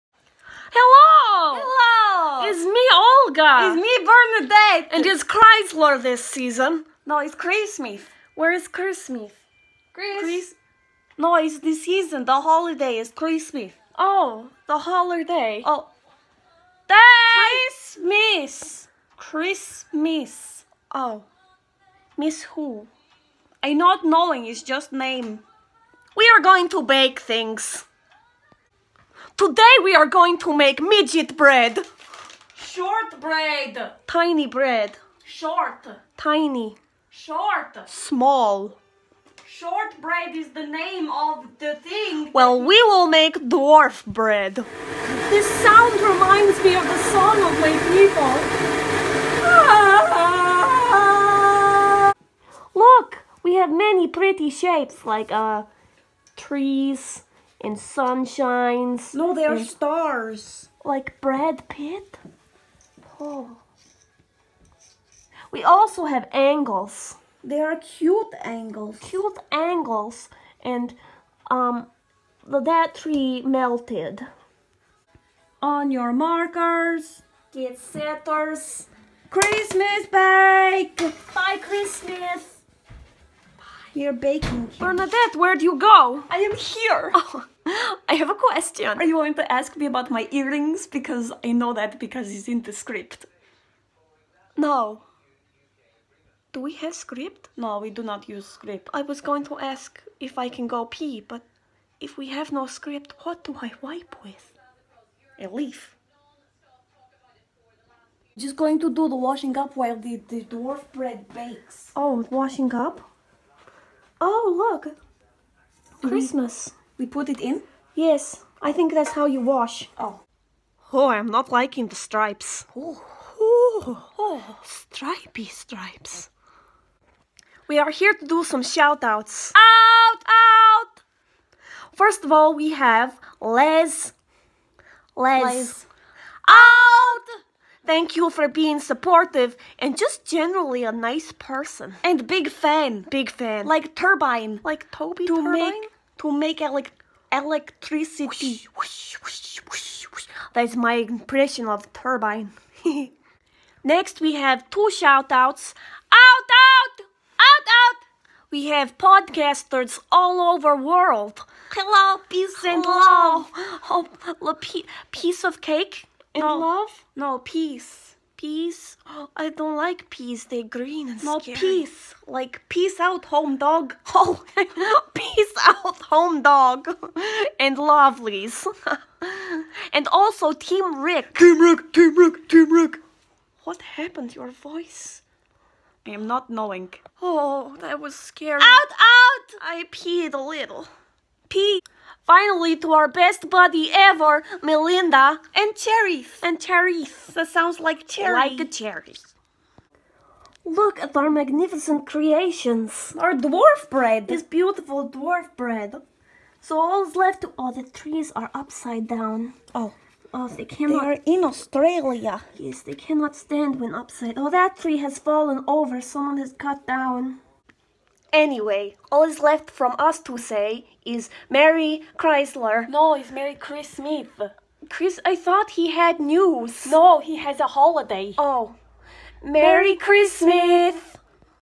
Olga and Bernadette CHRISTMAS BAKE OFF!
Olga and Bernadette are back for a special Christmas bake off! Complete with weird English, fires and Christmas things

Spread smiles!
Shine light in the darkness!
Make good choices!!

C&J